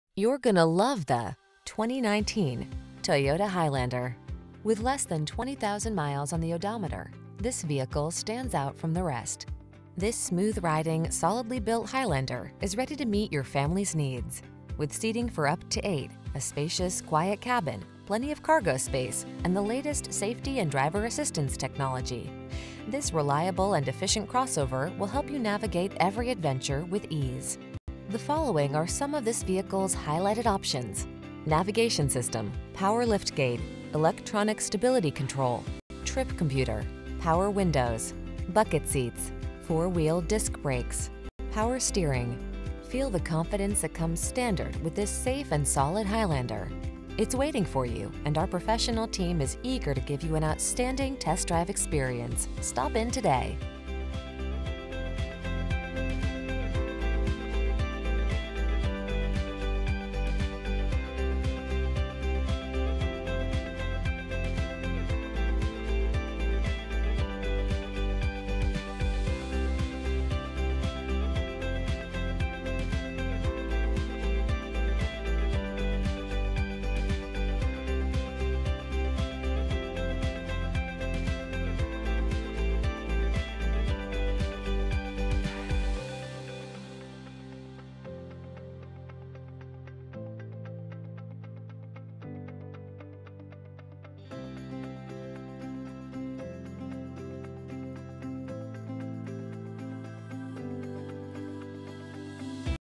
2019 Toyota Highlander at Oxmoor Toyota | Louisville & Lexington, KY U23288
Shoreline Blue Pearl Used 2019 Toyota Highlander available at Oxmoor Toyota in Louisville, Kentucky. Servicing the Lexington, Elizabethtown, KY New Albany, IN Jeffersonville, IN area.

Oxmoor Toyota
8003 Shelbyville Rd